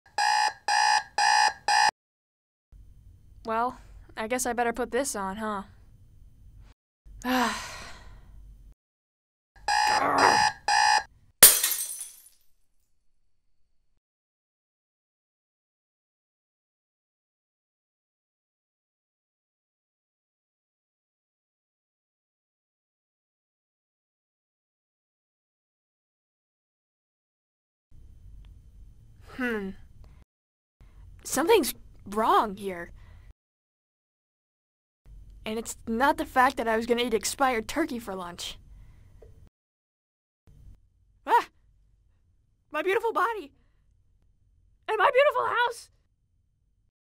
Dan Vs Fan Animated Cold Open WIP
A work in progress of a fan made dan vs cold open! the concept was as the video went on the quality would deteriorate, it was going to be Dan Vs Animation Major or something of the like! its very silly, but i would like to do something with it, and fix the long walk cycle ;w; still fun! Hope you like!
Character not mine!! Dan Mandel belings to the Hub show Dan Vs, created by Dan Mandel :)